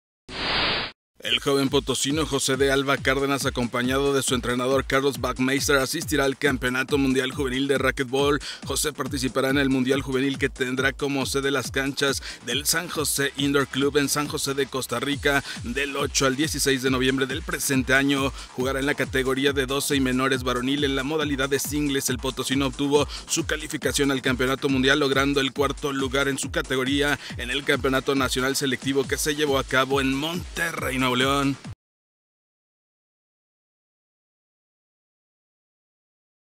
Califico el potosino José de Alba al Mundial Juvenil de Raquetbol.
SLP.- El joven potosino José de Alba Cárdenas acompañado de su entrenador Carlos Bacheaste asistirán al XXXI Campeonato Mundial Juvenil de Raquetbol.

José participará en el Mundial Juvenil que tendrá como sede las canchas del San José Indo Club, en San José Costa Rica del 8 al 16 de noviembre del presente año.

Jugará en la categoría 12 y menores varonil en la modalidad de singles, el potosino obtuvo su calificación al campeonato mundial, logrando el 4o. lugar en su categoría en el campeonato nacional selectivo que se llevó a cabo en Monterrey, Nuevo León.

Por: Eduardo Martínez Tapia.